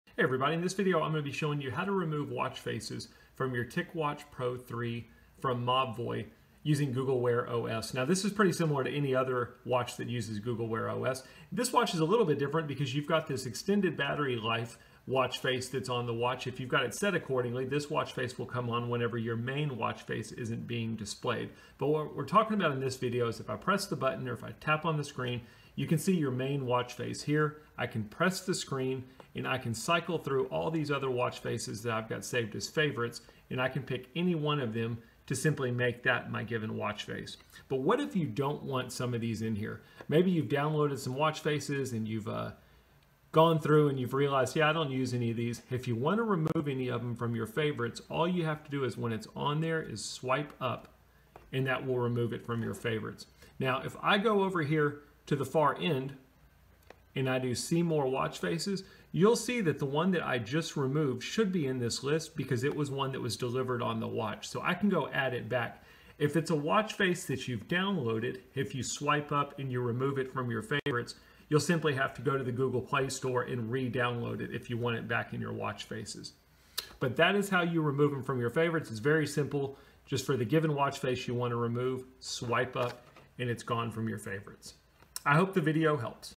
TicWatch Pro 3: Remove Faces from Favorites
TicWatch Pro 3 (by Mobvoi): How to remove faces from favorites